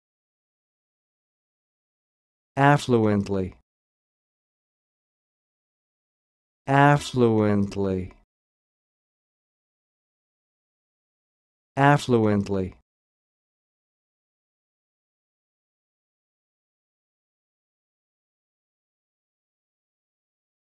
Learn how to say this word: "Affluently"
This is the definition of "Affluently":
adv.  Abundantly; copiously.